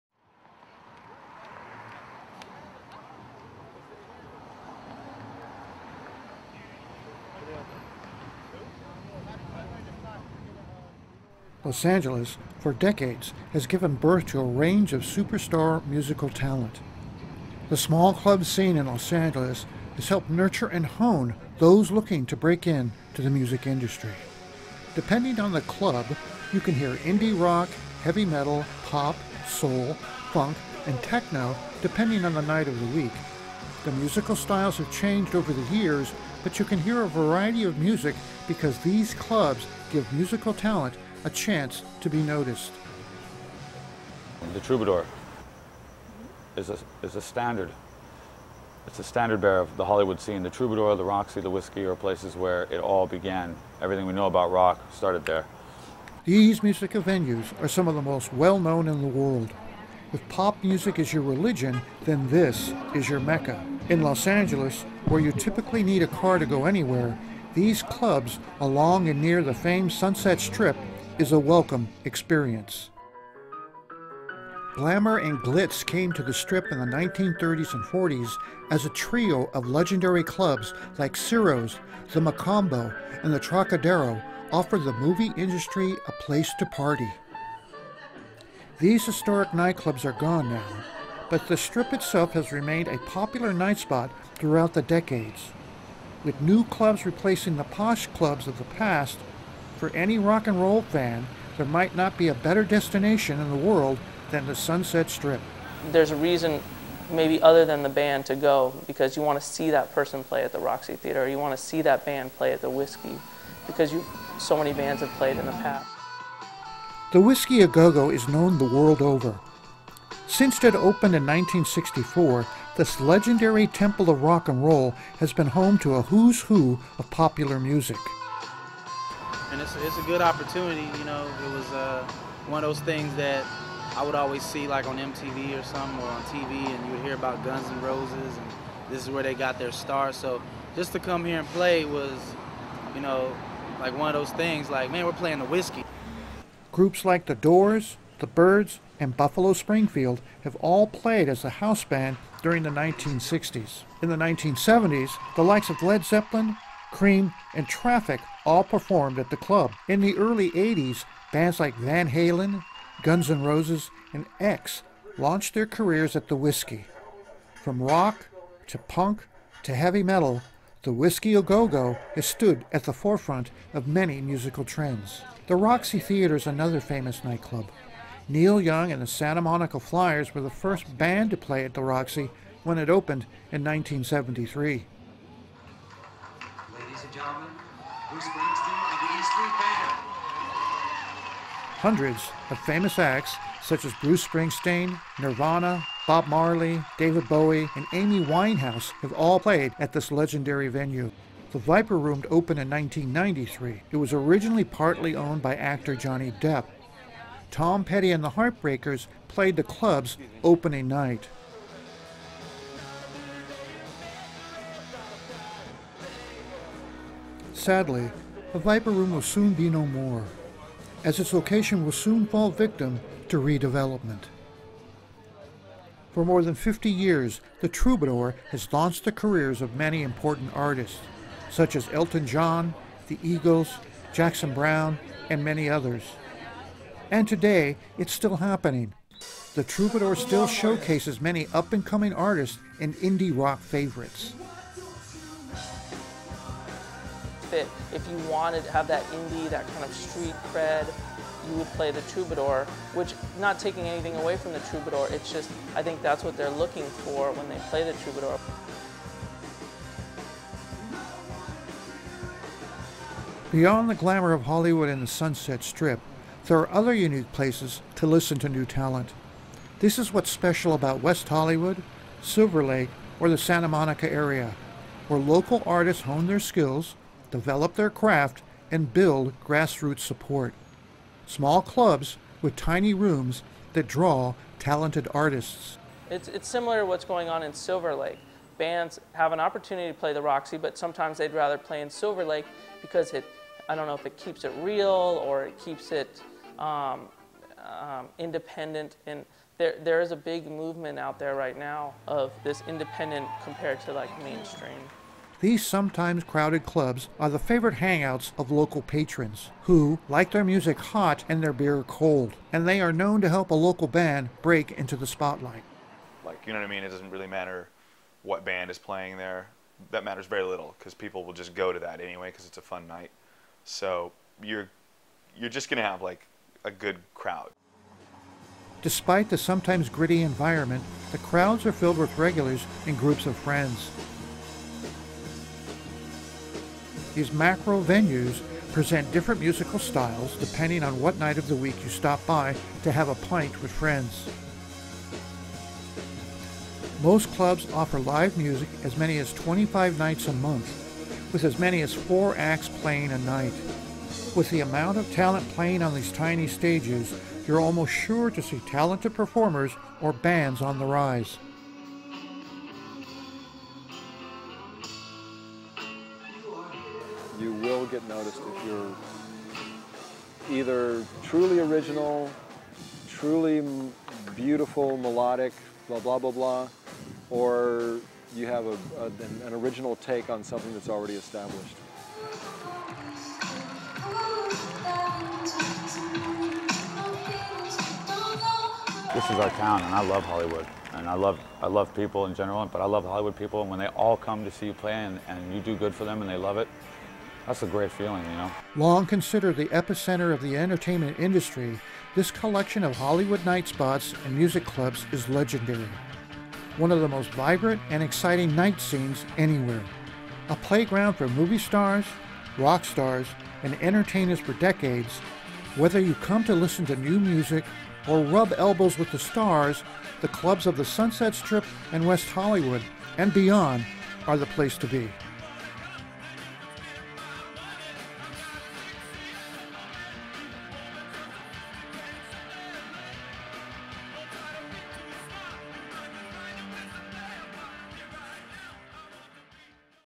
Best Small Music Clubs in Los Angeles
Los Angeles has for decades given birth to a range of super star musical talent. The Los Angeles small club scene has helped nurture and hone new musical talent.

Depending on the Club you can hear Indie Rock, Heavy Metal, Pop, Soul, Funk and Techno depending on the night of the week. The musical styles may have changed over the years but you can hear a variety of music because these clubs give musical talent a chance to be noticed.

These eclectic venues such as the Roxy Theater, The Viper Room, The Whisky a Go Go, and the Troubadour have serve up live music to packed houses for decades.

These music venues are some of the most well known in the world.  If pop music is your religion then this is your Meca.  In Los Aneles where you typically need a car to go anywhere, these Clubs along and near the famed Sunset Strip is a welcome experience.

Glamour and glits came to the Strip in the 1930s and 40s as a trio of legendary Clubs like Ciro’s, the Macambo and the Trocadero offered the movie industry a place to party.

These historic night clubs are gone now, but the Strip itself has remained a popular night spot throughout the decades. With new clubs replacing the posh clubs of the past.  For any rock and roll fan there might not be a better destination in the world than the Sunset Strip.

The Whisky a Go Go is known the world over.  Since it opened in 1964, this legendary temple of Rock and Roll has been home to a who’s who of popular music.

Groups like the Doors, The Birds and Buffalo Springfield have all played as the house band during the 1960s.    In the 1970s the likes of Led Zeppelin, Cream and Traffic all preformed at the Club.

In the early 80s band like Van Halin, Guns N’ Roses, and "X" launched their careers at the Whisky. From Rock, to Punk to Heavy Metal, The Whisky a Go Go has stood at the forefront of many musical trends.

The Roxy theater is another famous night club.  Neil Young and the Santa Monica Flyers were the first band to play the Roxy when it opened in 1973.

Hundreds of famous acts such as Bruce Springsteen, Nirvana, Bob Marley, David Bowie and Amy Winehouse have all played this Legendary venue.

The Viper Room opened in 1993.  It was originally partly owned by Actor Johny Depp.  Tom Petty and the Heartbreakers played the Club's opening night.  Sadly, the Viper Room will soon be no more, as it’s location will soon fall victim to redevelopment.

For more than fifty years The Troubadour has launched the careers of many important artist.  Such as Elton John, The Eagles, Jackson Brown and many others.  And today that is still happening.  The Troubadour still showcases many up and coming artists and Indie Rock favorites.

But beyond the glamour of Hollywood and the Sunset Strip there are other unique places to listen to new talent.  This is what is special about West Hollywood, Silverlake, or Santa Monica areas, where local artist horn their skills, develop their craft and develop grass roots support.  Small clubs with tiny rooms that draw talented artist.

These sometimes crowed clubs are the favorite hangouts of local patrons,  who like their music hot and their beer cold. And they are known to help a local band into the sport light.

Despite the sometimes gritty environment, the crowds are filled with regulars and groups of friends.  These micro venues presents different musical styles, depending on what night of the week you stop by to have a pint with friends.

Most Clubs offer live music as many as twenty-five nights a month with as many as four acts playing a night. With the amount of talent playing on these tiny stages, you are almost sure to see a talented performer or band on the rise.

Long considered the epicenter of the entertainment industry this collection of Hollywood night spots and music clubs is legendary.  One of the most vibrant and exciting night life scenes anywhere.

A play ground for movie starts, rock stars, and entertainers for decades. Whether you come to listen to new music or just to rub elbows with the stars, the clubs of the Sunset Strip, West Hollywood and beyond is the place to be.